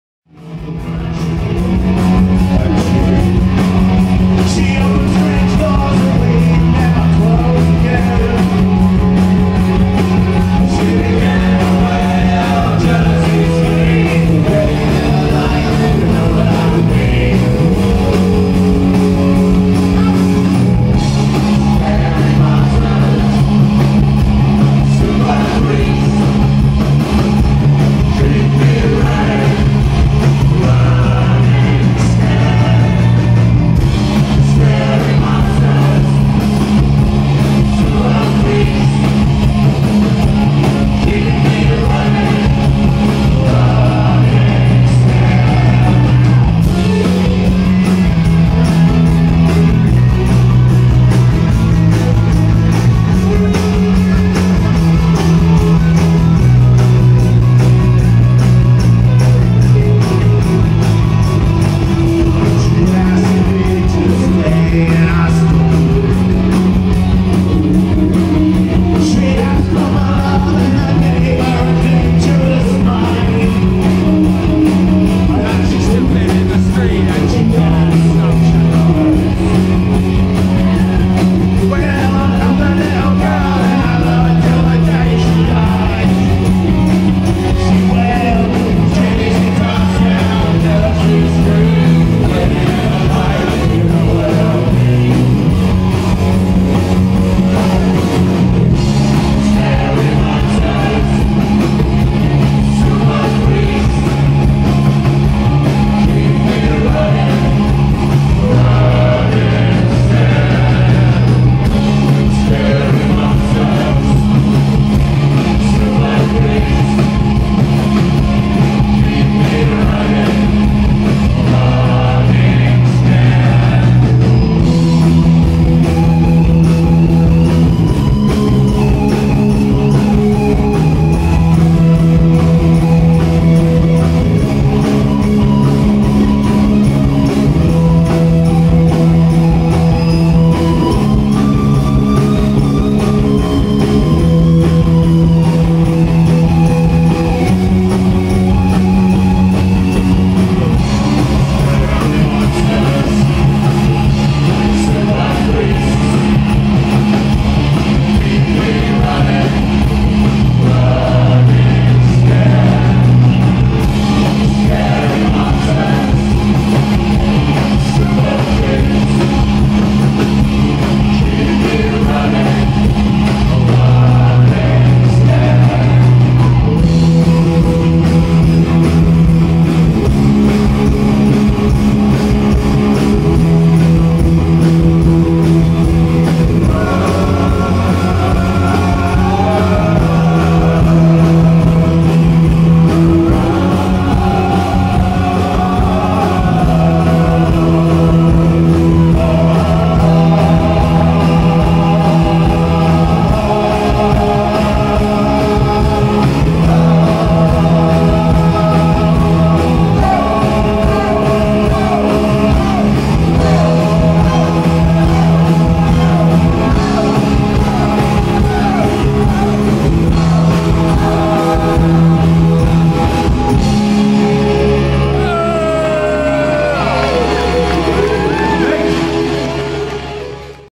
National People's Gang
At Bowie tribute night, Saturn, Birmingham, AL Jan 29th, 2016.